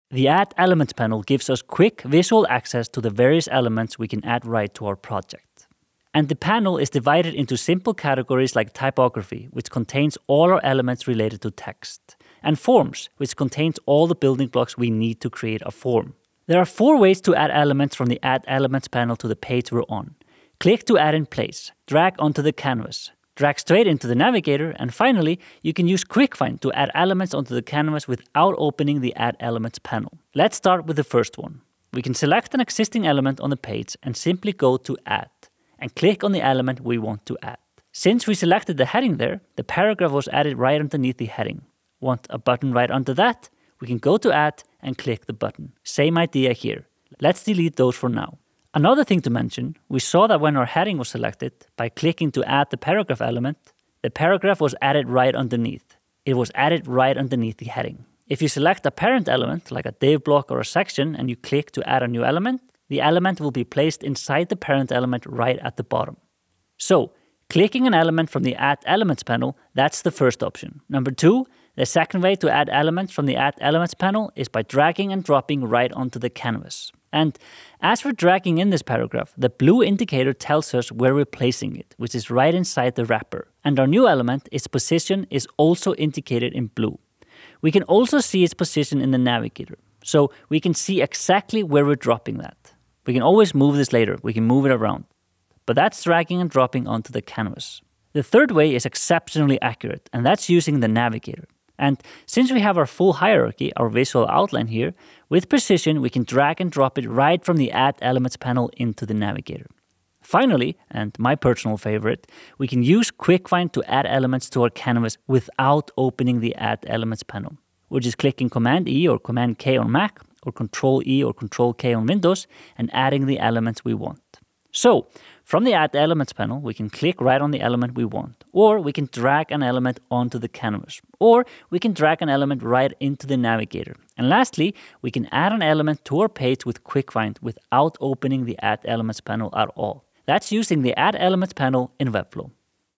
Add Elements panel — Webflow UI tutorial
Webflow's Add Elements Panel gives you quick, visual access to the various elements you can add to your project, from layout elements like divs to complex prebuilt components likes sliders. In this video, we'll cover the structure of the Elements Panel, and how to add elements in the following steps:

00:00 — Introduction
00:30 — Click to add in place
01:06 — Drag onto the canvas
01:35 — Drag into Navigator
01:52 — Add using quick find
02:04 — Recap